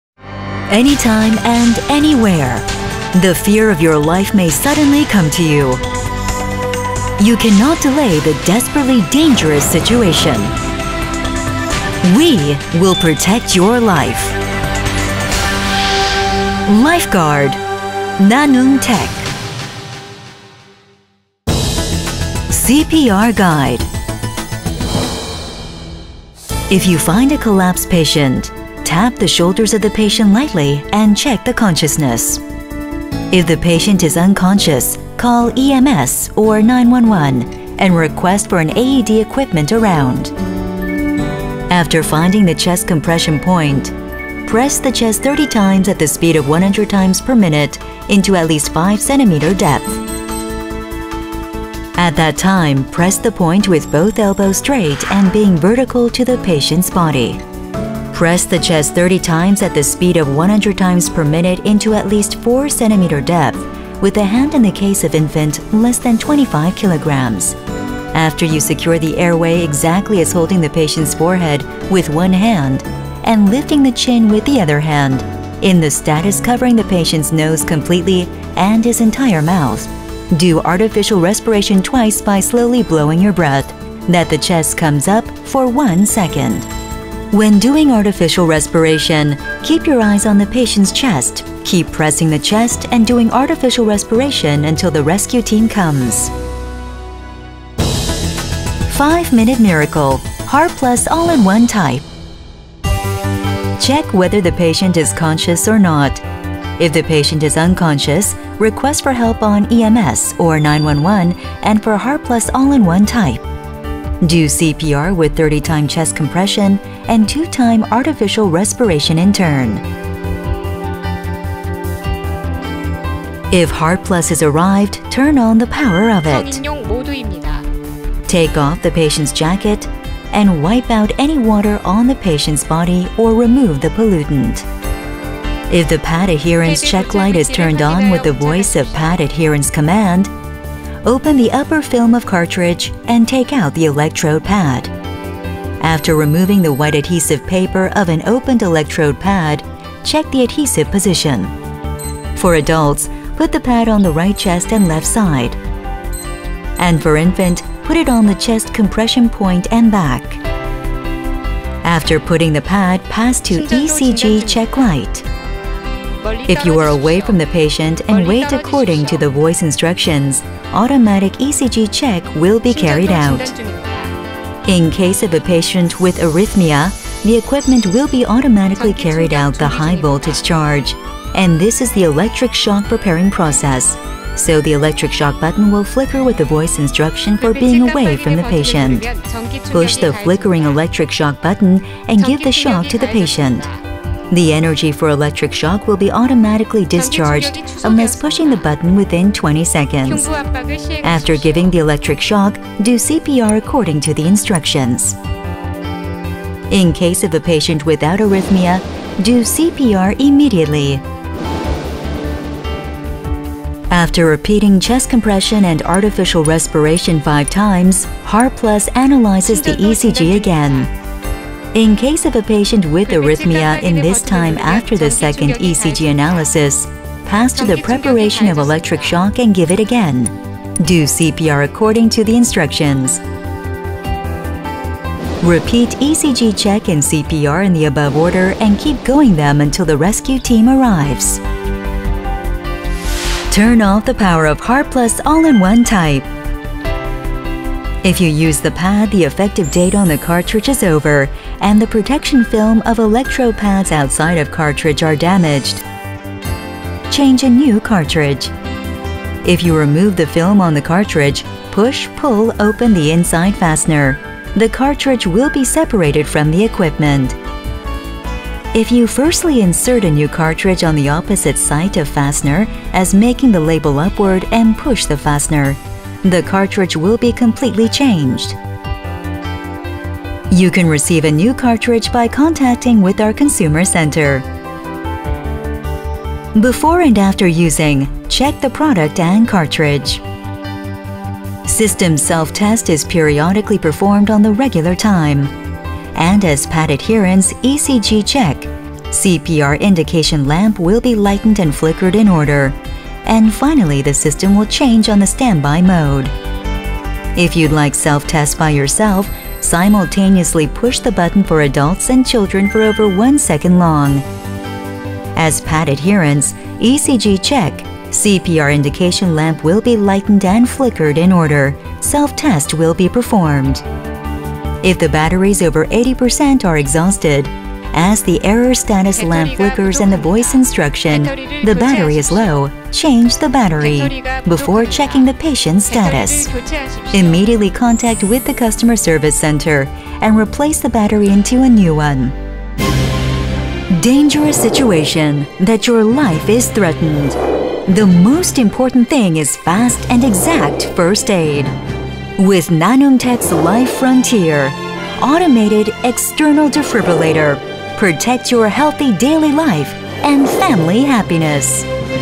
HeartPlus AED, Live saving machine!
HeartPlus AED is an life saving device to deliver an electric shock to the patient. Everybody can use easily HeartPlus AED on power on HeartPlus AED, You just can follow the voice prompt.Don't be embarrased an emergence situation, Just look for the HeartPlus AED.